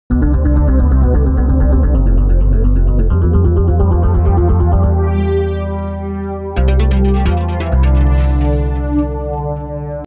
Canal Cinco 10 anos - ID Cubos V3 (2018)
Com grandes estreias e muitas outras coisas mais, a rede Canal Cinco deu início às comemorações por seus ¡10 anos! no ar. A 21 de Abril de 2008 nasceu esta gigante rede de televisão fictícia espanhola - provavelmente a maior em língua portuguesa - sob o nome de Rede Ituran. O nome atual foi adotado em 2011.
O novo pacote gráfico da emissora é uma adaptação do ano anterior, com inúmeros aperfeiçoamentos.